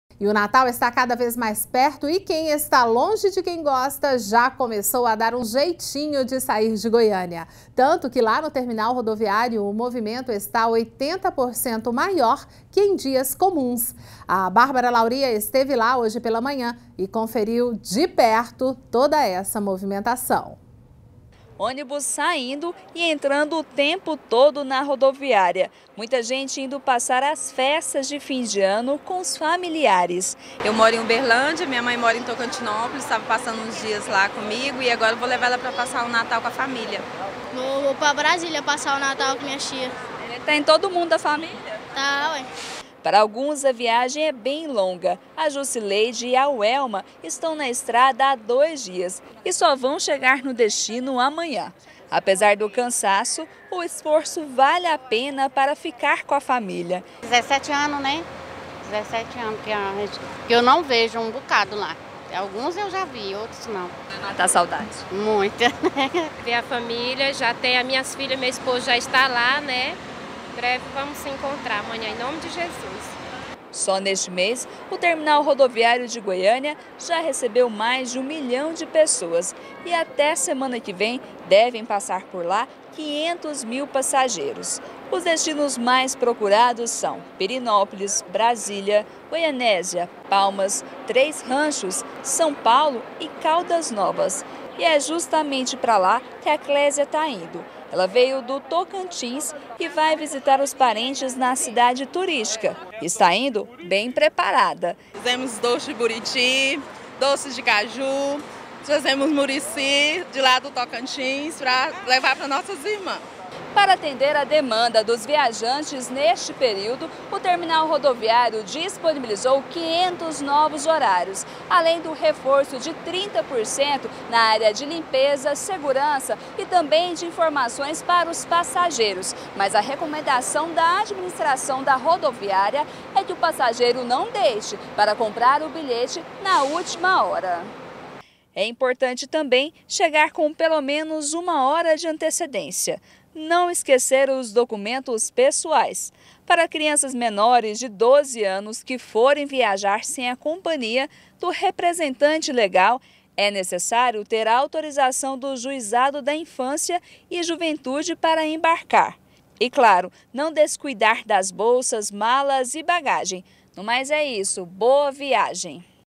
Movimento na rodoviária está 80% maior do que em dias comuns
Só no mês dezembro a rodoviária de Goiânia recebeu mais de 1 milhão de pessoas. Os destinos mais procurados são Pirenópolis, Brasília, Goianésia, Palmas, Três ranchos, São Paulo e Caldas Novas.
INFORMAÇÕES:
Assista: